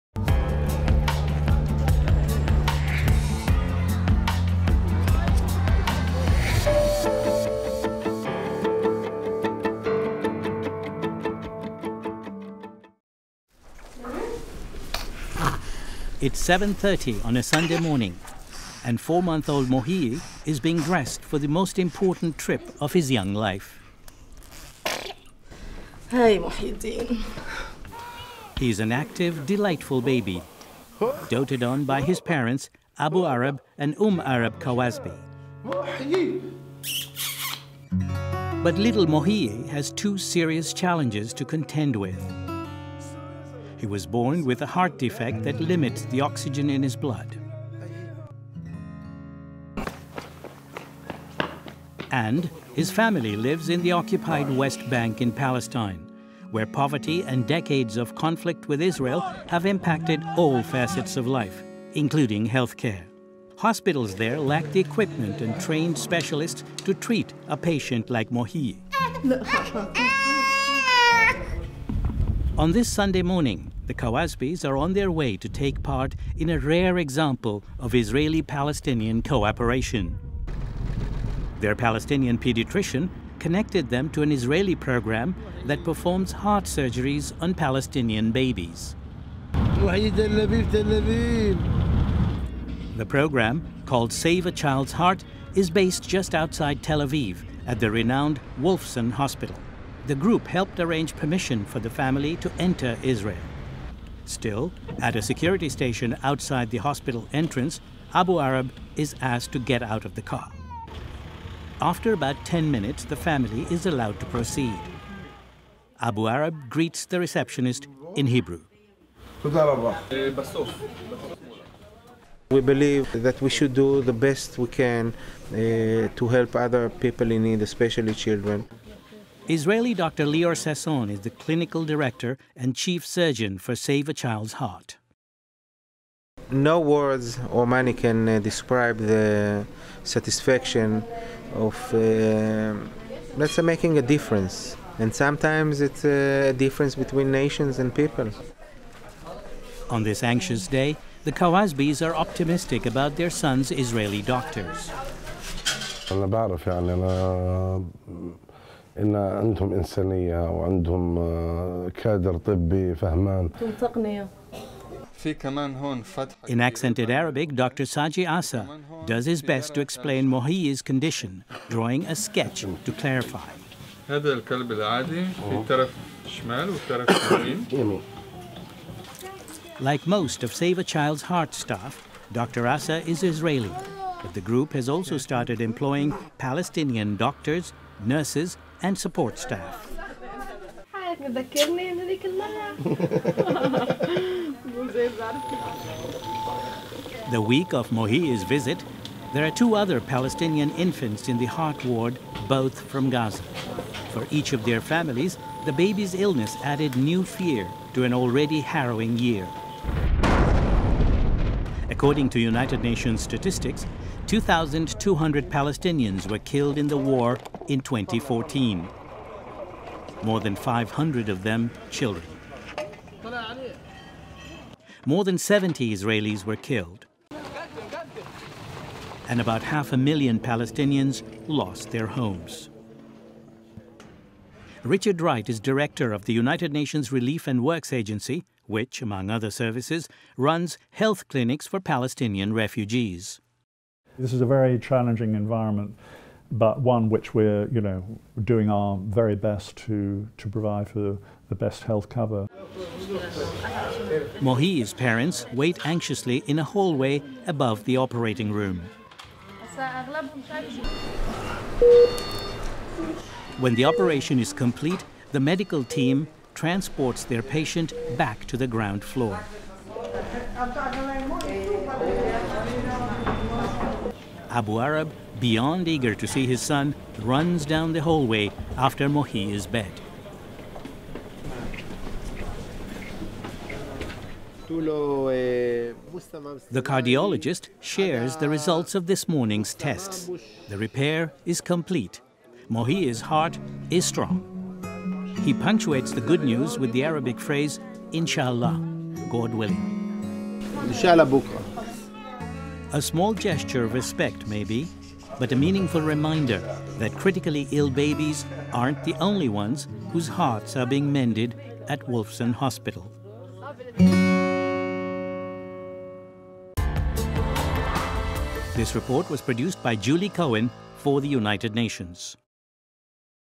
Palestina e Israel: Consertando pequenos corações
Este é provavelmente o pior pesadelo dos pais: um bebê extremamente doente e nenhuma ajuda médica. Uma parceria envolvendo Israel e a Palestina não apenas está salvando vidas de crianças, mas também ajudando a promover o entendimento mútuo. Confira nesta matéria da TV ONU.